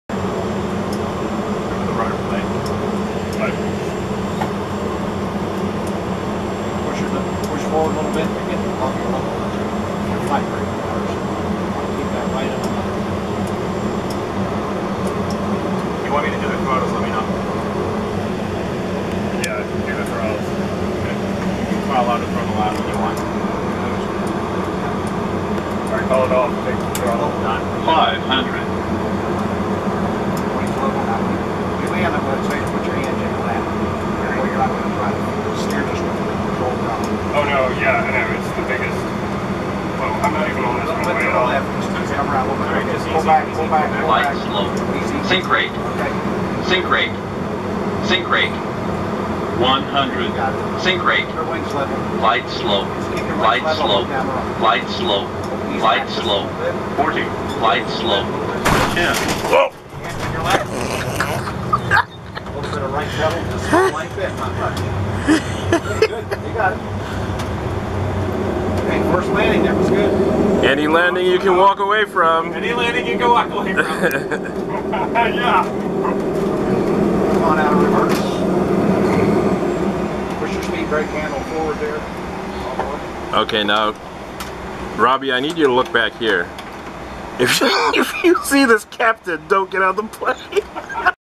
Delta Airlines 737-200 Full-motion Flight Simulator [HD] - May 21, 2016
Captain Mathis "lands" at JFK.